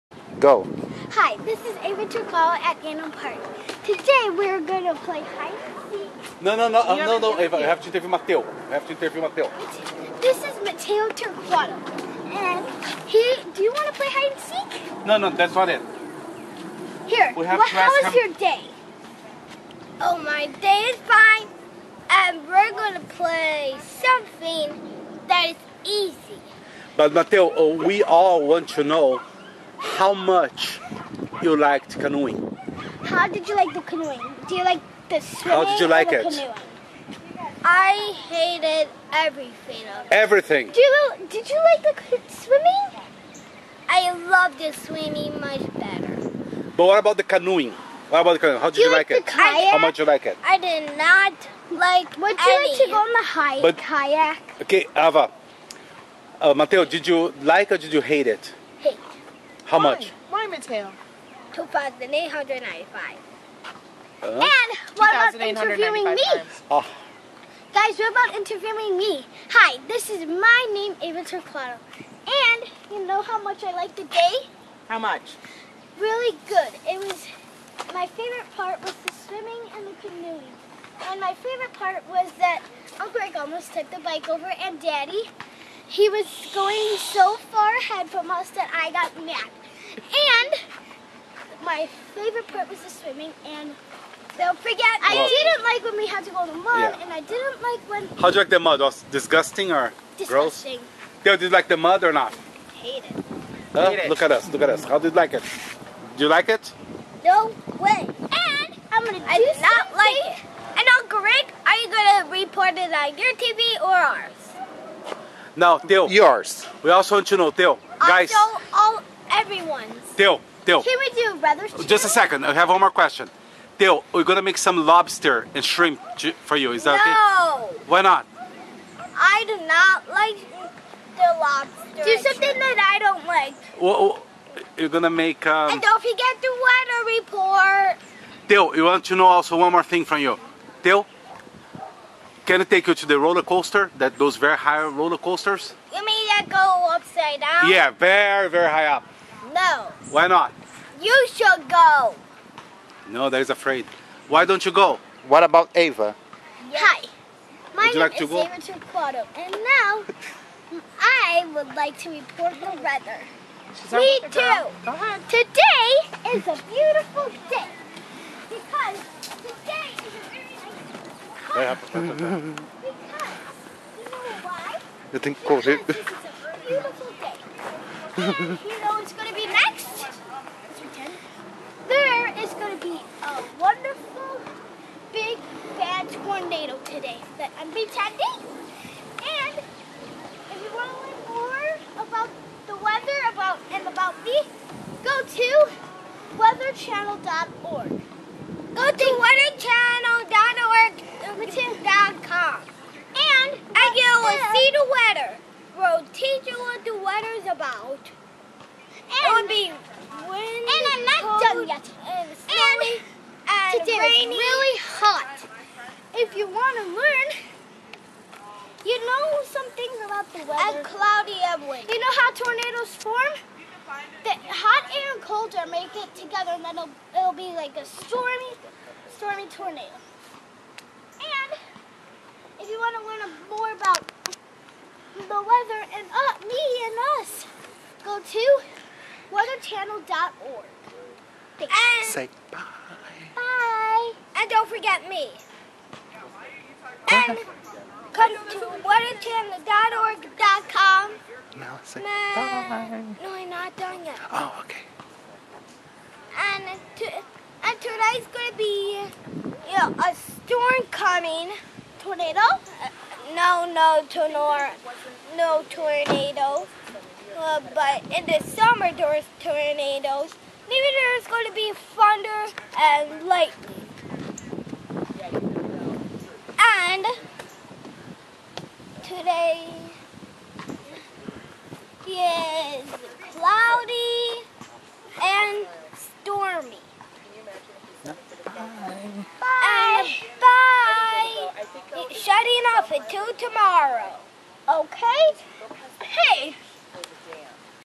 Canoe Day
Matteo Talking About his Canoe Day.